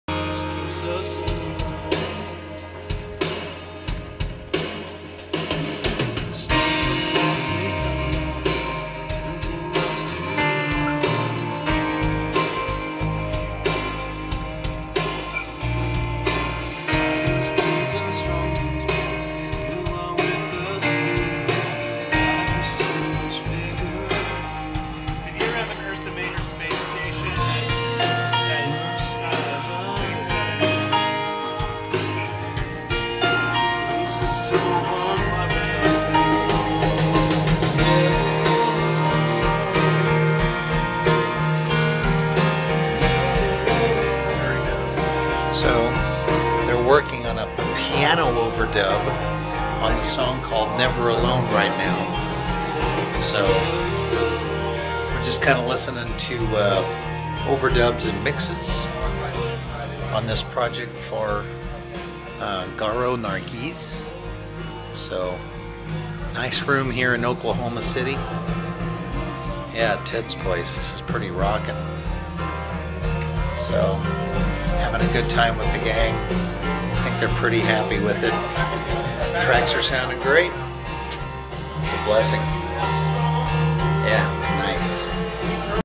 Carl Albrecht / Garo Nargiz session / listening to trax.
In the control room listening while the piano player works on an overdub.